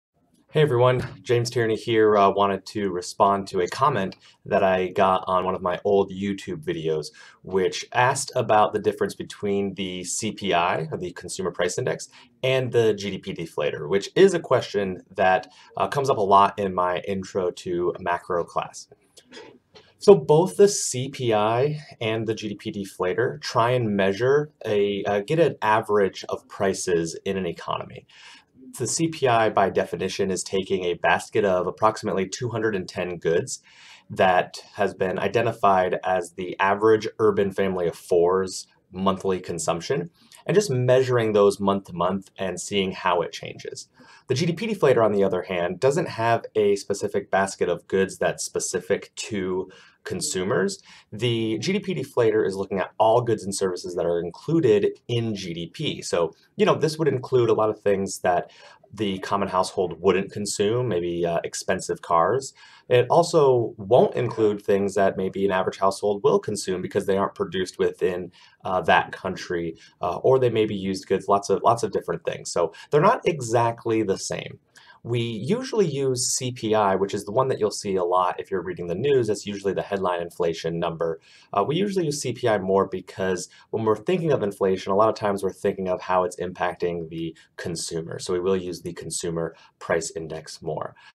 The Difference Between the CPI and GDP Deflator (Explained!)
An explanation of some macroeconomic indicators to help econ students succeed.

A user commented on an old video of mine titled, "Calculating the Inflation Rate with CPI Values saying, "Btw, I heard that cpi and gdp deflator were that same, is that right?"

This is not completely true. They both are indeed used to calculate the inflation rate as they are both  price indexes. However, what goods and services they are using are different. The video explains.

Here is a good write-up by the BLS going more in-depth on the difference

I hope this video helps you become the best student you can be this semester.

You can learn more about my private tutoring and academic coaching by visiting